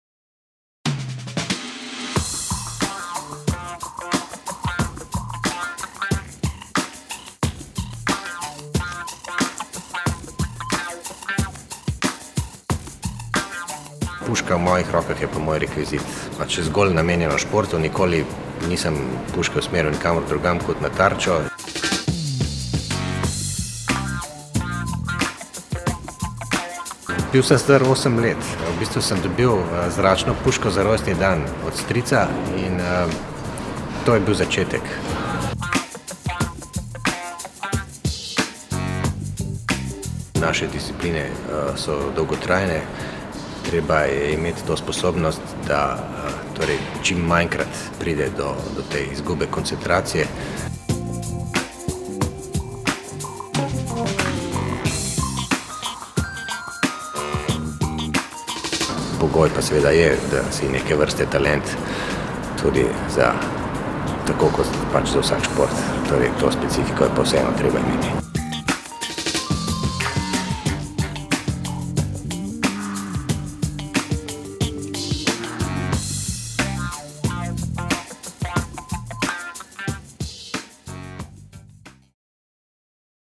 Rajmond Debevec, olimpijski pravk v športnem streljanju
Rajomnda Debevca smo ujeli na strelišču, kjer nam je pokazal, kako izgleda njegov trening in spregovoril o tem, kaj odlikuje dobrega strelca.